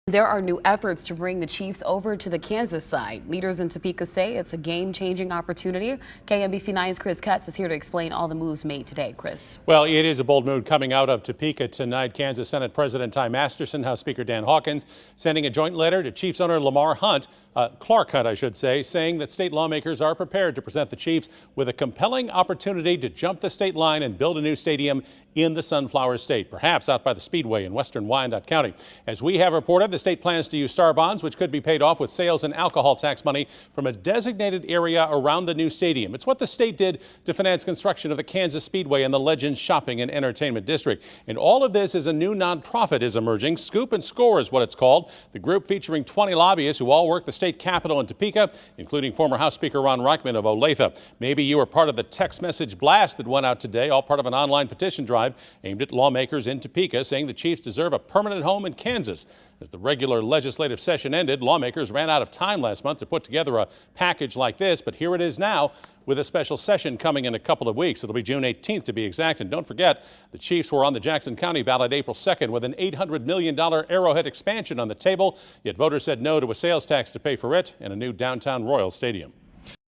'A game-changing opportunity': Kansas Senate President, House speaker reach out to the Chiefs
The efforts to bring the Kansas City Chiefs across state lines to Kansas took another turn on Tuesday.